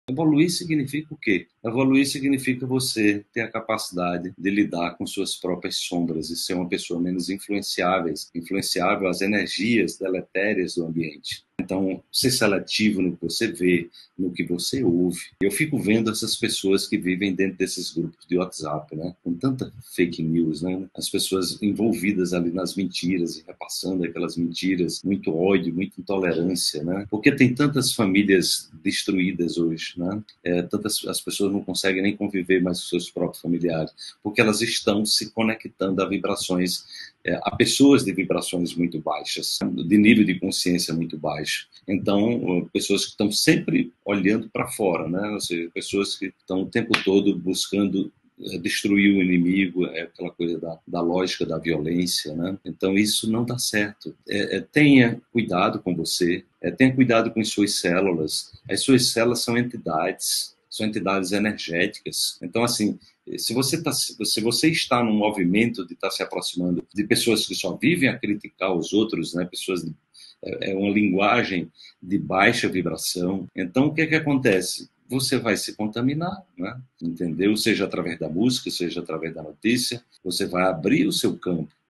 Evoluir significa o que? | Wallace Lima
Tenha muito cuidado com aquilo que você ouve, com o que você vê e fala.

É fundamental ser seletiva(o), escolher bem os ambientes, as pessoas e os assuntos com os quais você está lidando.

Se você deseja evoluir, é preciso qualificar o seu diálogo interior e se proteger das suas próprias negatividades e das pessoas e ambientes carregados.

Conecte-se a conteúdos, informações e pessoas de níveis de consciência mais Elevados.
Se você deseja curar a sua vida, é preciso elevar a sua vibração e
mudar padrões de comportamento negativos.

No Próximo sábado, dia 04 de março, das 17h às 21h, juntamente com Evelline Carvalho, estaremos realizando o Workshop Proteção Energética e Espiritual na Prática.